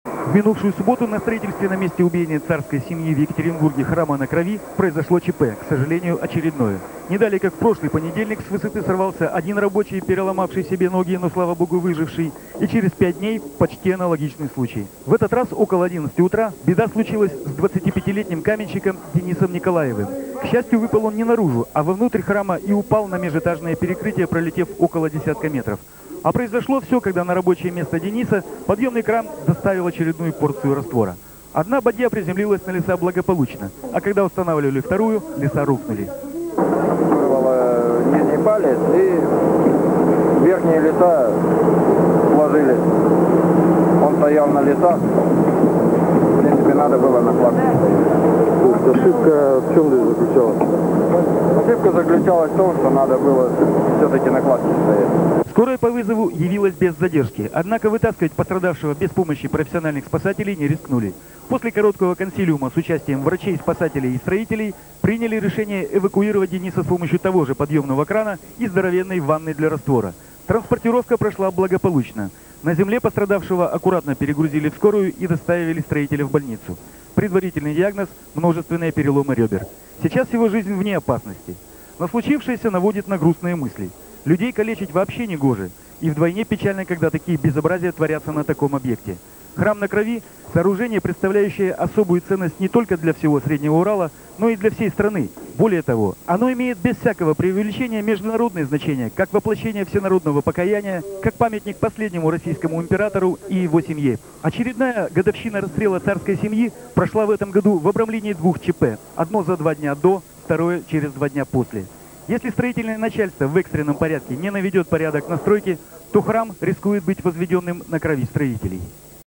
Каменщик рёбра переломал, возводя Храм-на крови
Жирные00-нулевые. Новости 22 июля 2002 года